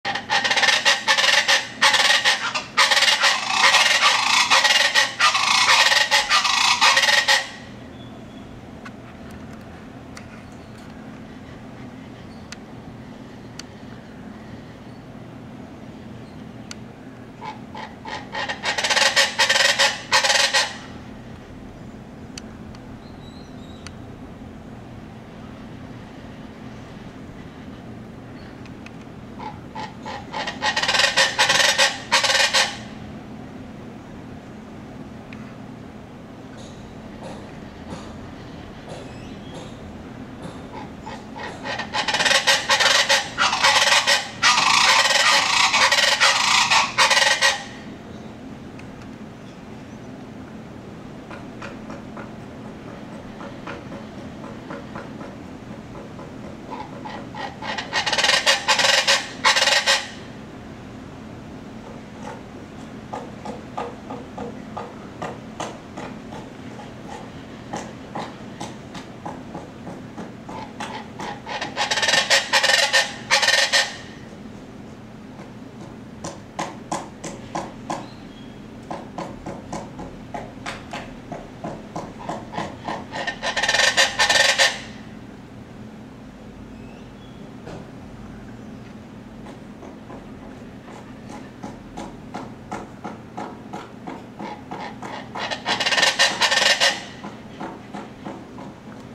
Chachalaca Huatulco
How do they make that horrible sound ? Do they have to do it at 6:30 AM ?